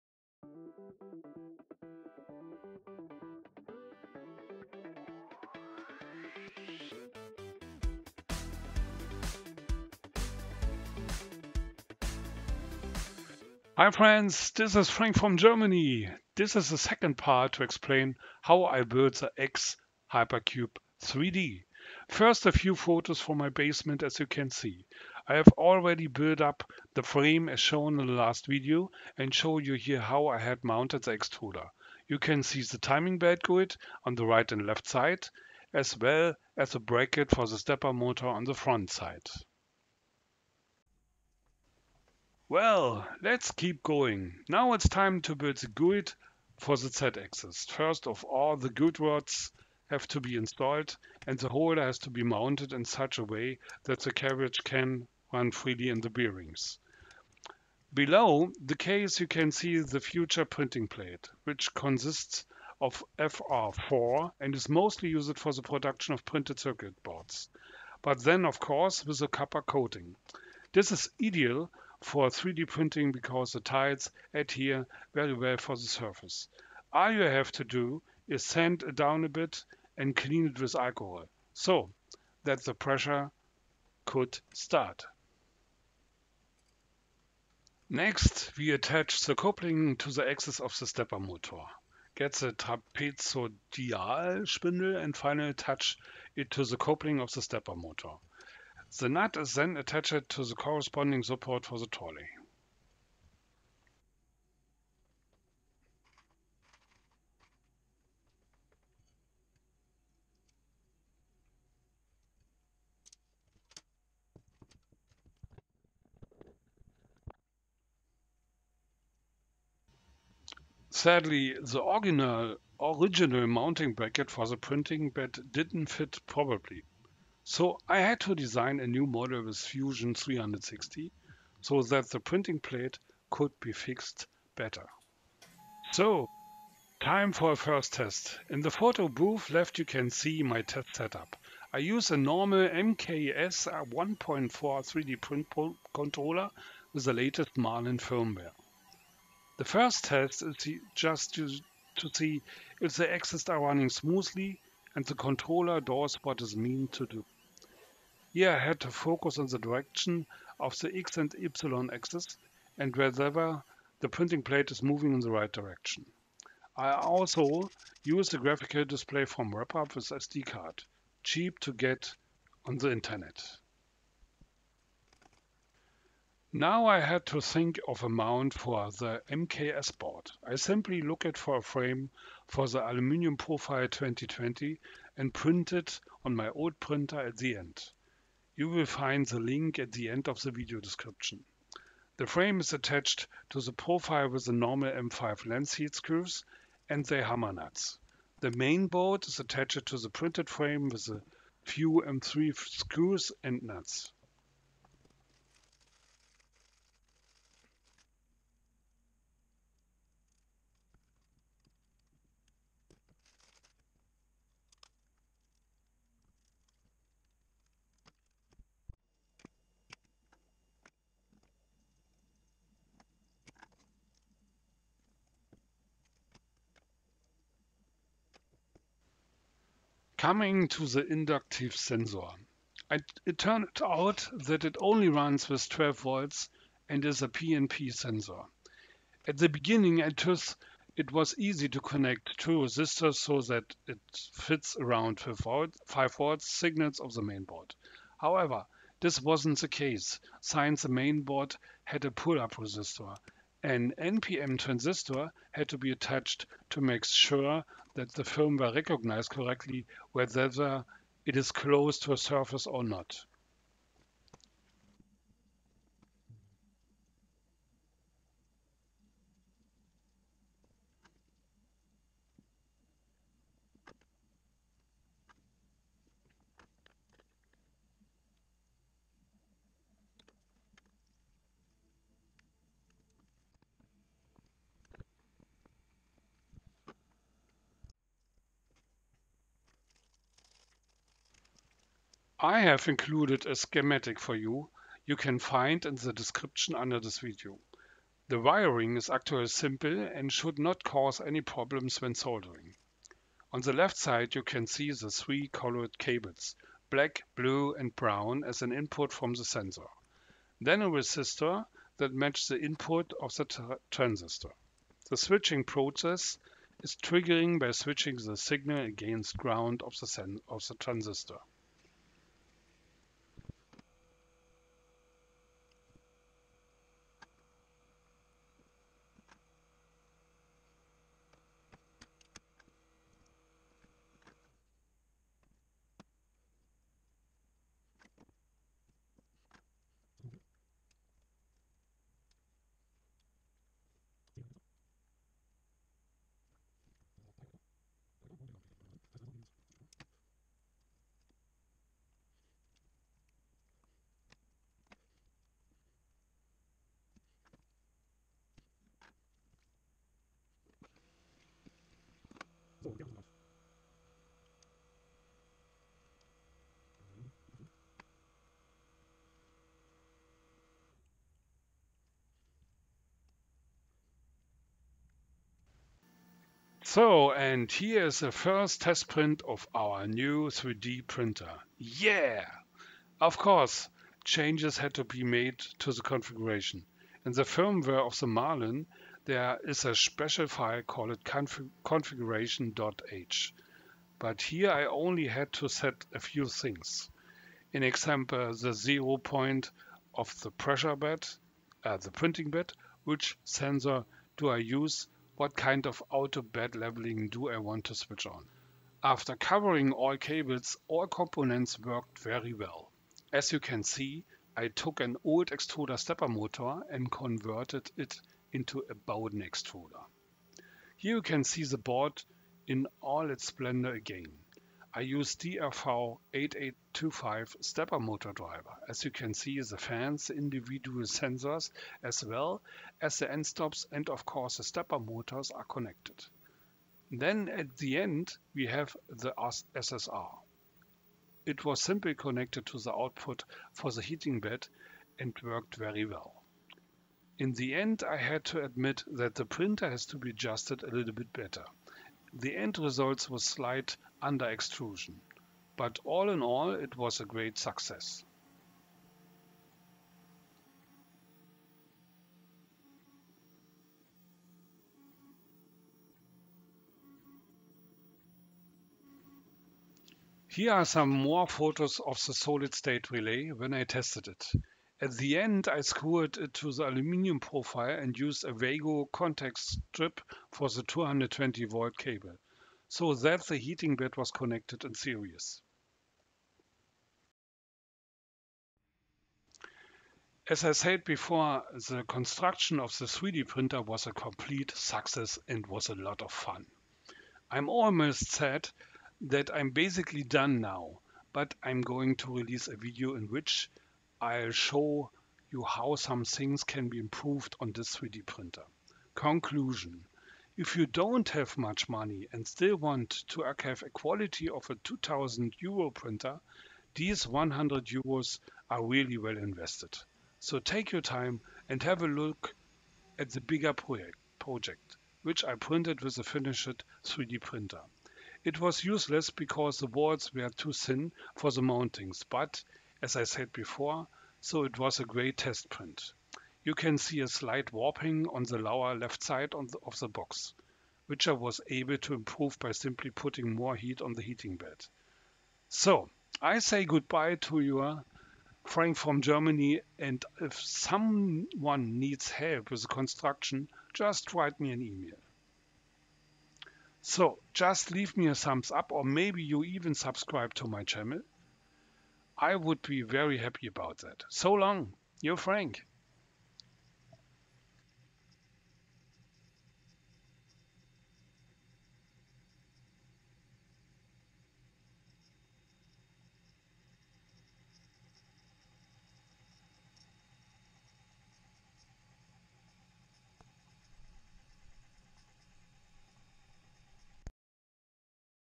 S01E02 Build a Hypercube 3D Printer
Hi friends, this is Frank from Germany. This is the second part to explain how I built the XHypercube 3D printer.